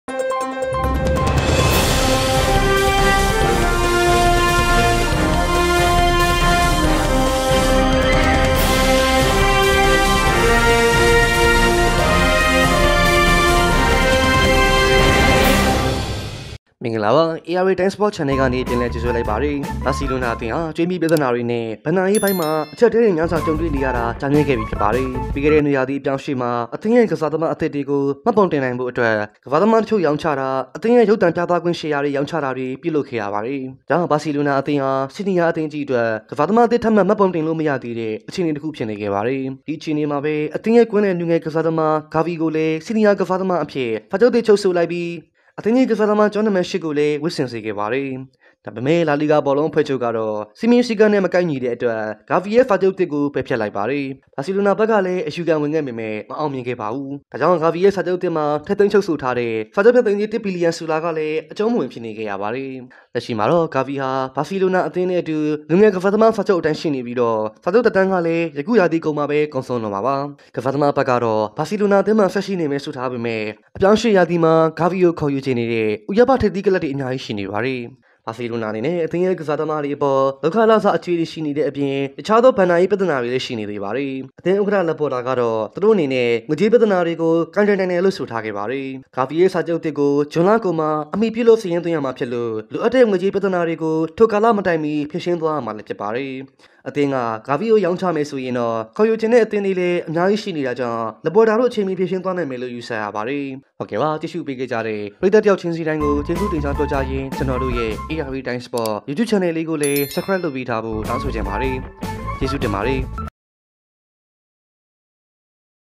ဘာစီလိုနာနဲ့ ဂါဘီရဲ့ စာချုပ် အရှုပ်တော်ပုံ
ဒီနေ့ တင်ဆက်ပေးသွားချင်တဲ့ အကြောင်းအရာကတော့ ဘာစီလိုနာအသင်းနဲ့ အသင်းရဲ့ လူငယ်ကစားသမား ဂါဘီတိုရဲ့ စာချုပ်အရှုပ်တော်ပုံ အကြောင်းပဲ ဖြစ်ပါတယ်။

ဘာစီလိုနာအသင်းဟာ ကြွေးမြီပြဿနာတွေနဲ့ ဘဏ္ဍာရေးပိုင်းမှာ အကြပ်အတည်းများစွာကြုံတွေ့နေရတာ ကြာမြင့်နေပြီဖြစ်ပါတယ်။ ပြီးခဲ့တဲ့ နွေရာသီ အပြောင်းအရွေ့မှာ အသင်းရဲ့ ကစားသမားသစ်တွေကို မှတ်ပုံတင်နိုင်ဖို ကစားသမားတစ်ချို ရောင်းချတာ ၊ အသင်းရဲ့ ရုပ်သံပြသခွင့် ရှယ်ယာတွေရောင်းချတာတွေ ပြုလုပ်ကာ ဆေးမြီးတိုနည်းနဲ့ ဖြေရှင်းခဲ့ပါတယ်။